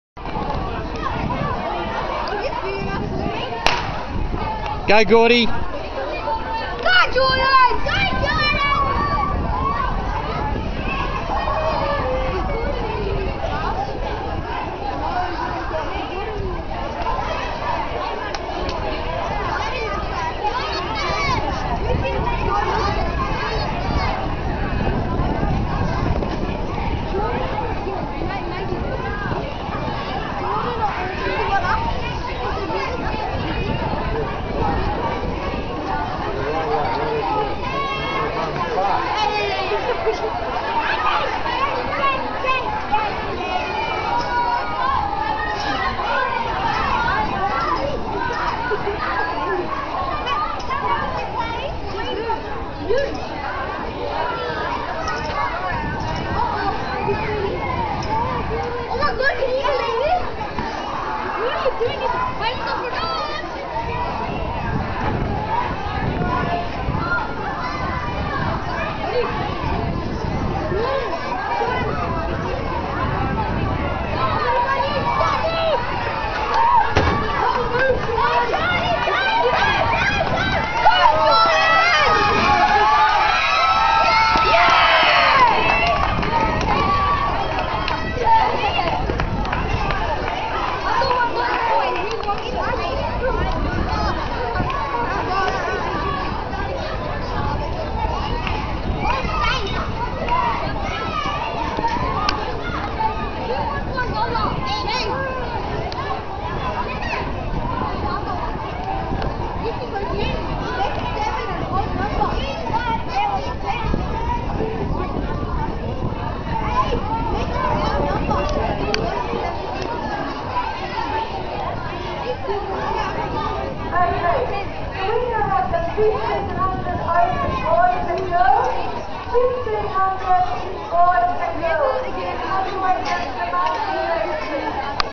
JordanF - 400m Winner Combined Greek Orthodox 2011
Jordan win the Junior 400m at the 2011 Combined Greek Orthodox Athletics Carnival.

Other results for the day 800m 1st, 100m 3rd, 200m 3rd, Long Jump 2nd & Under 9's Relay Team First.